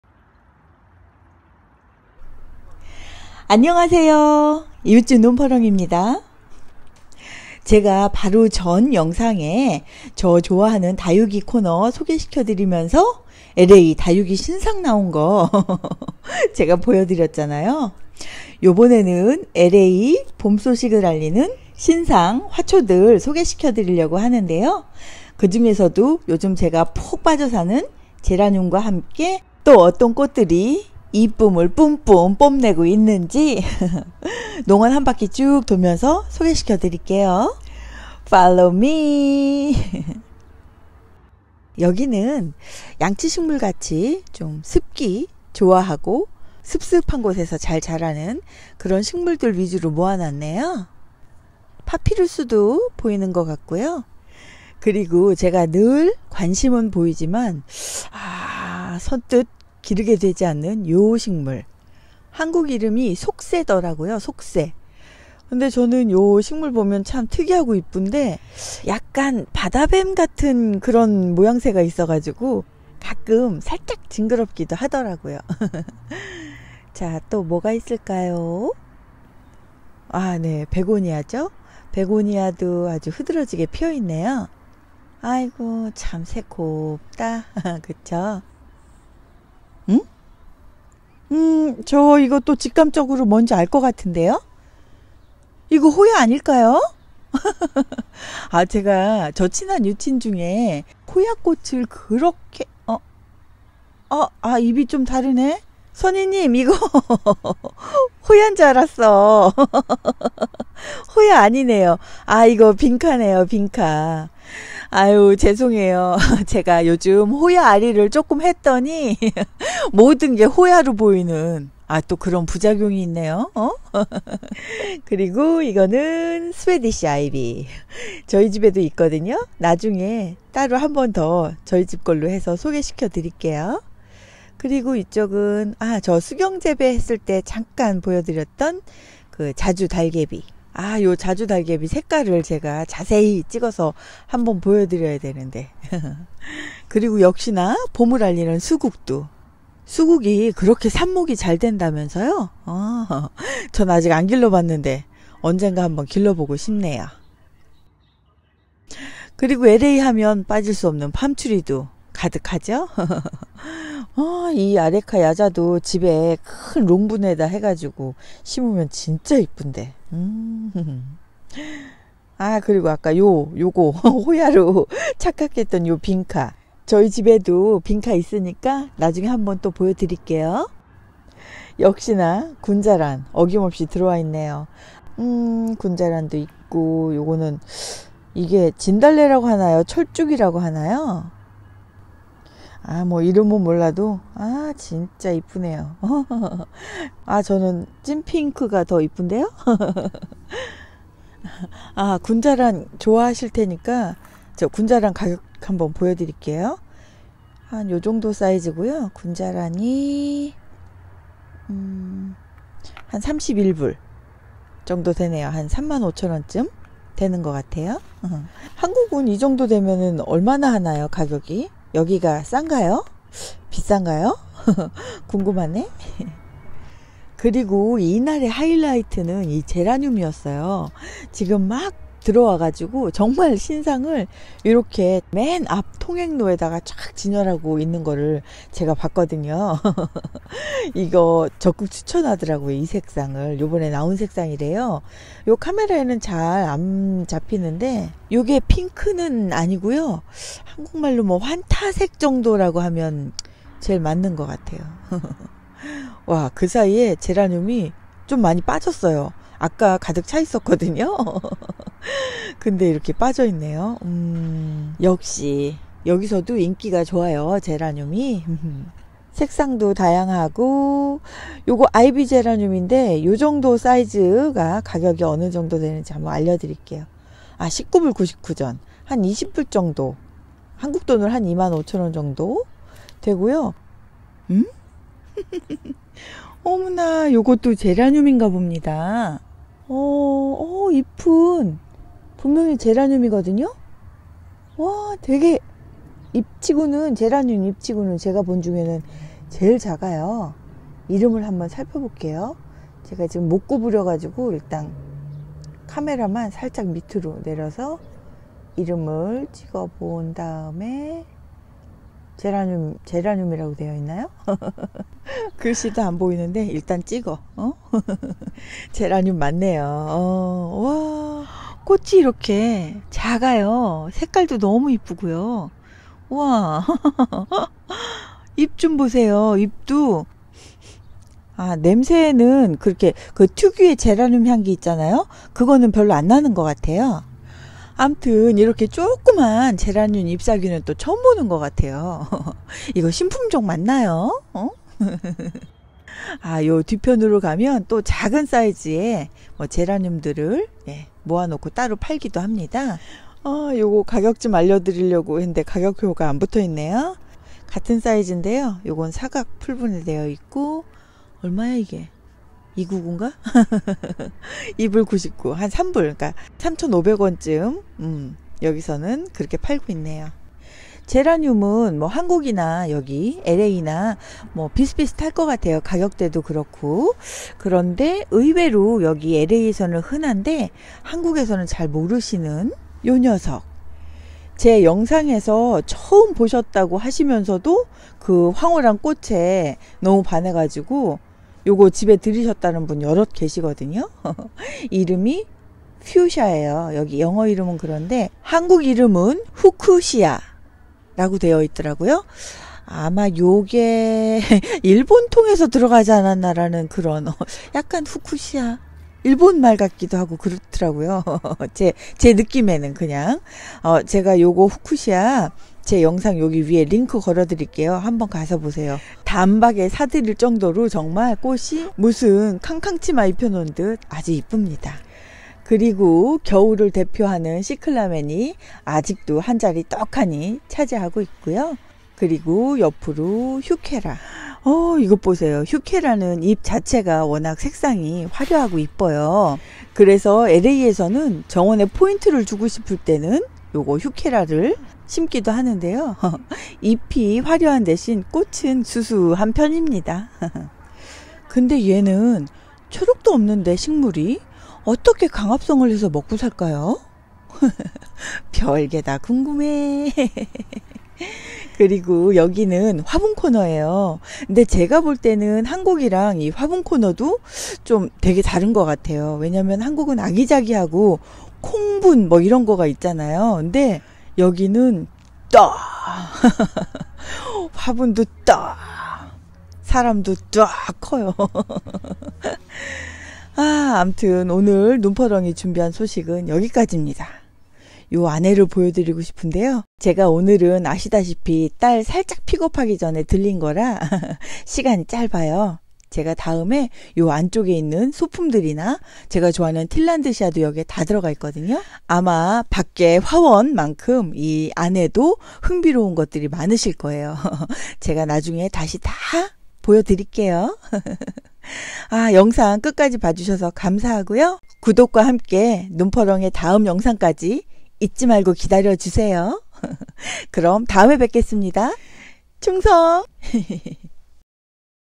요번엔 새로나온 LA 신상 화초들~ 꽃들~ 보여드릴거예요!! 한국 화원이랑 다른게 있을까요??^^ Spring Flowers
엘에이에 사는 눈퍼렁~!!^^
바로 전 영상에  새로나온 LA신상 다육을 보여드렸다면...
요번엔 2020년 엘에이에 봄을 알리는 화초들이 뭐가 있는지??
보여드릴려고 영상 준비했습니다.

얼릉얼릉~ 구경하러 오세요~!!ㅎㅎ